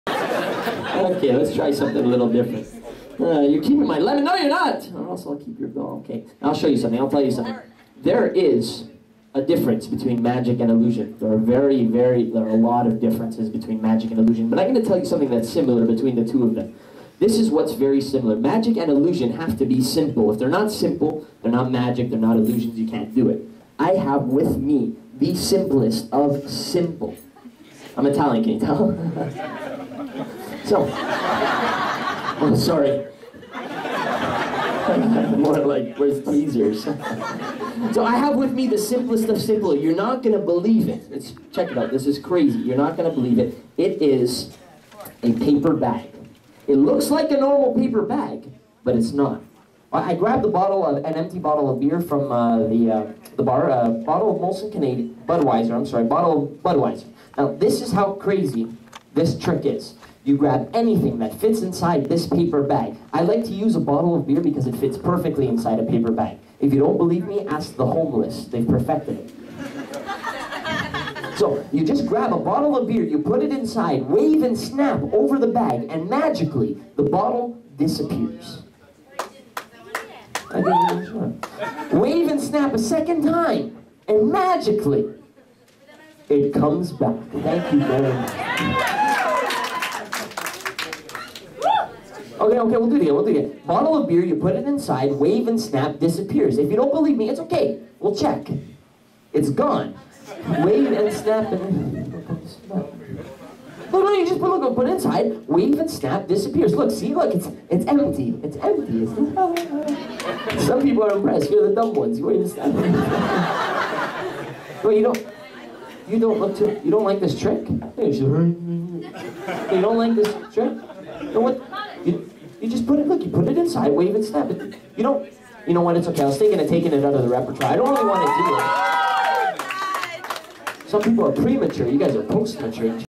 Revel Magic - Beer in a bag
Internet Take Over Productions Presents
Robert Testa, Revel Magic Performing at Fox and the Fiddle in Toronto.